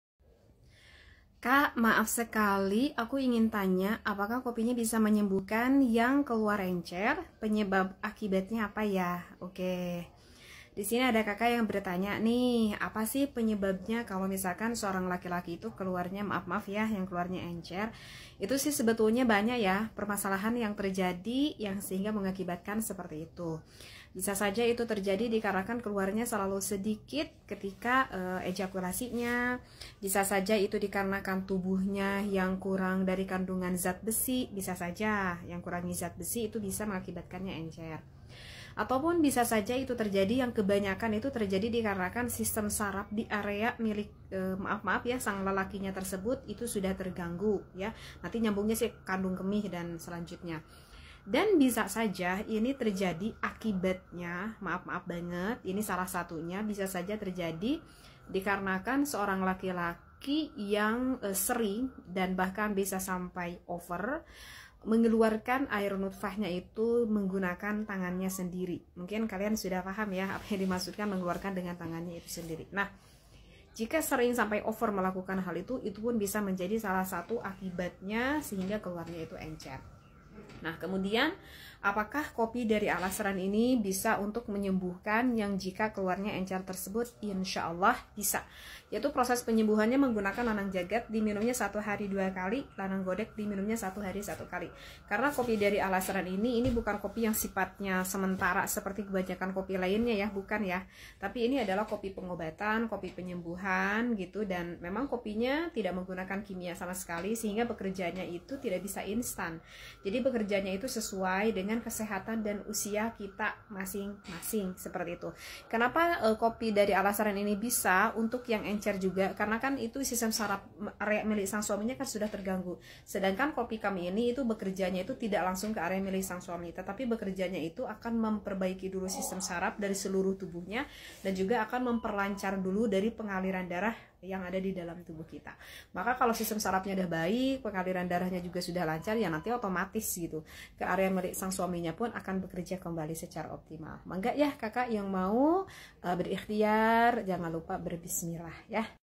KOPI AL-ASRAN INSYAA ALLOH DAPAT MENGOBATI M*NI YG ENCER
apabila kalin mengalami keluhan m*ani yg encer,cobalah berusaha melalui meminum kopi al asran,

Insyaa Alloh kopi Al asran dapat mengobatinya,
dg catatan sabar dalam proses nya,

iarena kopi al asran tidak mengandung kimia dan prises nya tidak instan,

SEMOGA BERMANFAAT BUAT KEBAHAGIAAN PASUTRI ANDA